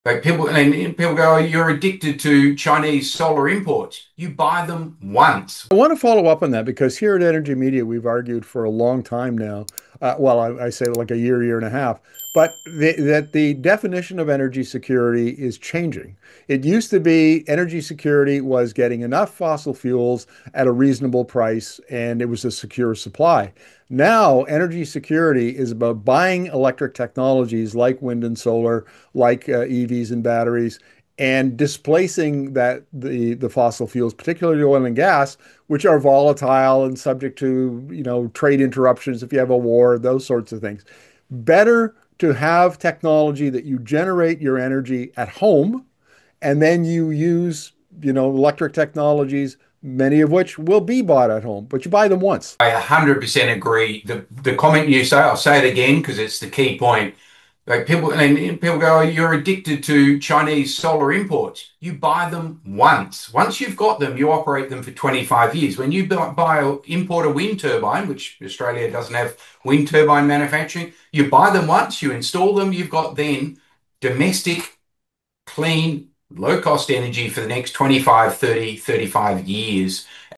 “You Buy Them Once”: Why Clean Tech Is the New Energy Security
Clean energy isn’t a dependency — you buy the tech once and get decades of stable, low-cost power. Solar, wind, EVs and batteries are redefining energy security by replacing volatile fossil fuels with domestic, reliable tech.